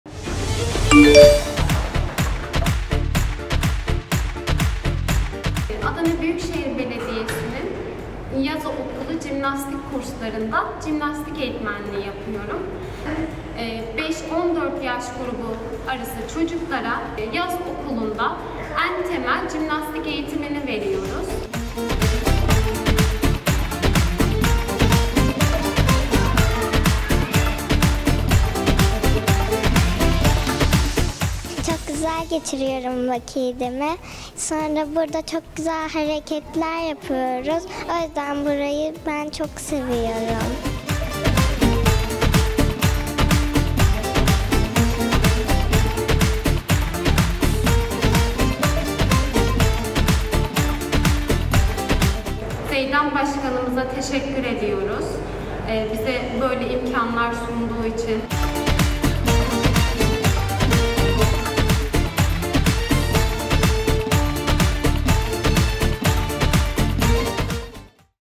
Yaz Spor Okulları Jimnastik 🤸🏻‍♀️
5-14 yaş aralığındaki çocuklar uzman eğitmenlerimizden aldıkları eğitimleriyle fiziksel ve zihinsel gelişimlerine katkı sağlıyor.